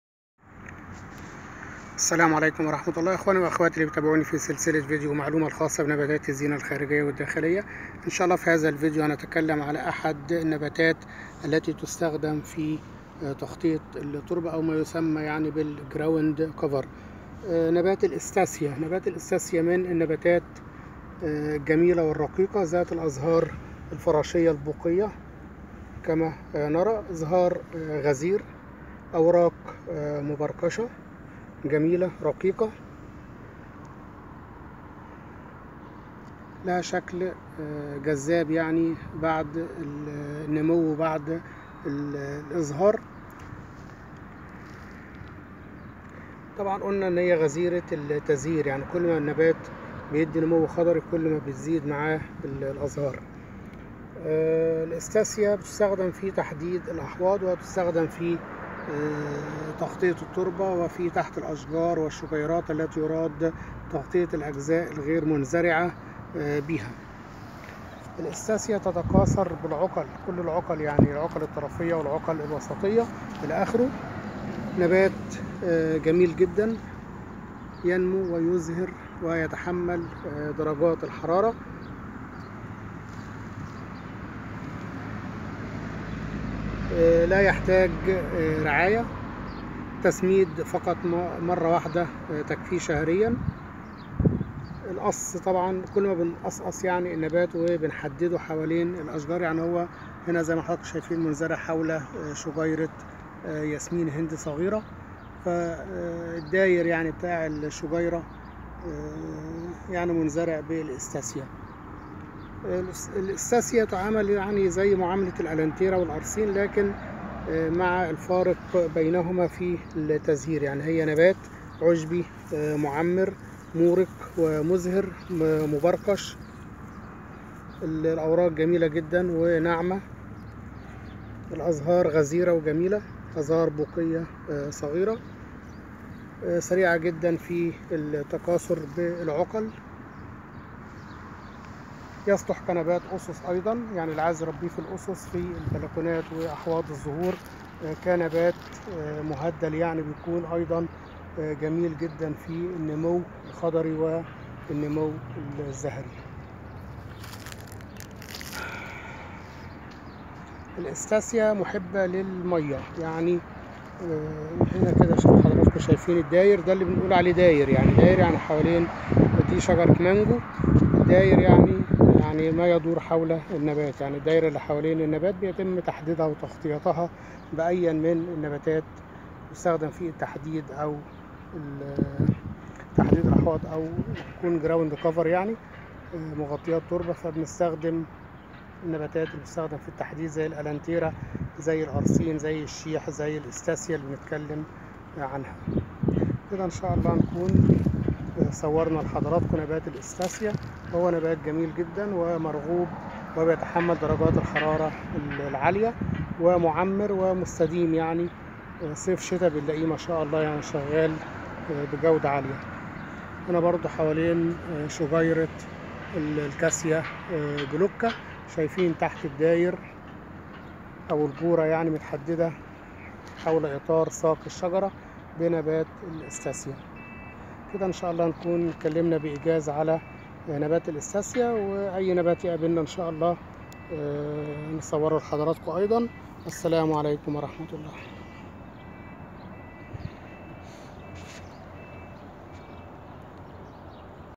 نبات الاستاسيا
نبات عشبى معمر مفترش يتكاثر بالعقل من نباتات التغطية والتحديد